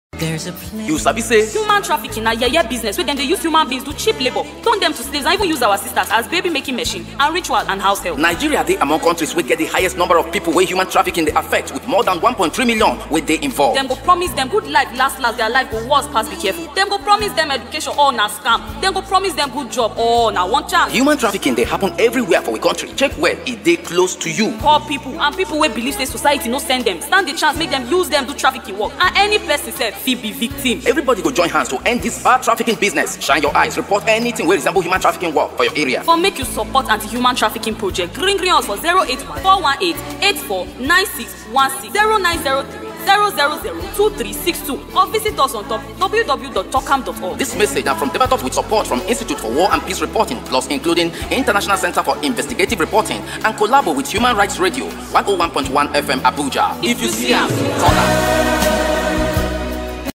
What You Need To Know About Human Trafficking in Nigeria
Human trafficking happens in every state in Nigeria.